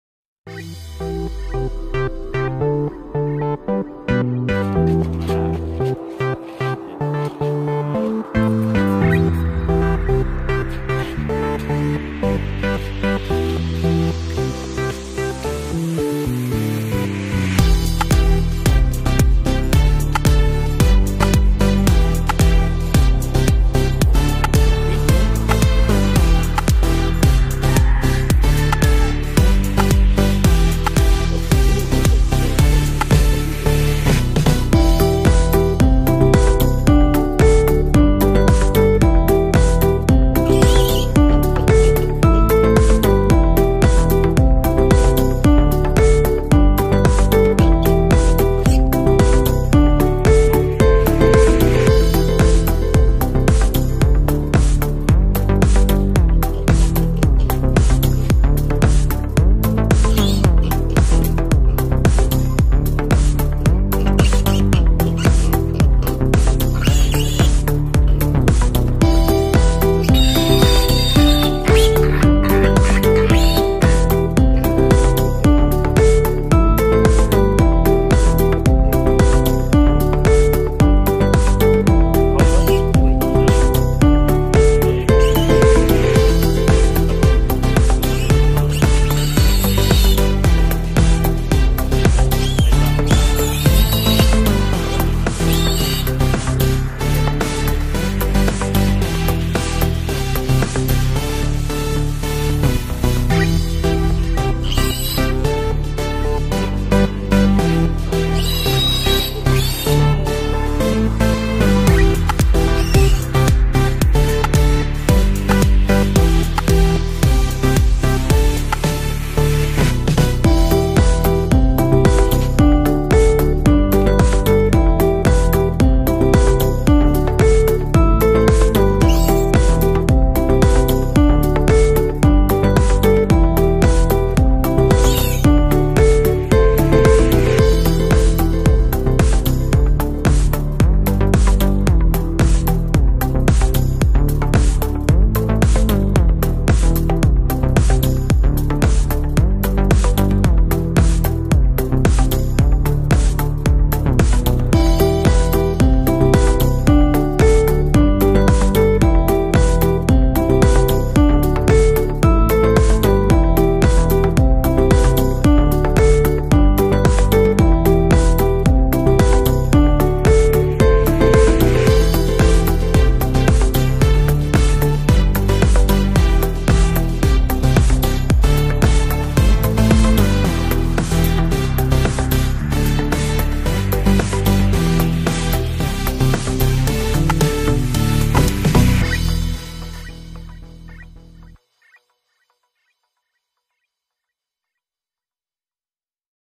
HOW TO CASTRATE A PIGLET
Castration is a common management tool in hog raising for many reason one is to stop the production of male hormones.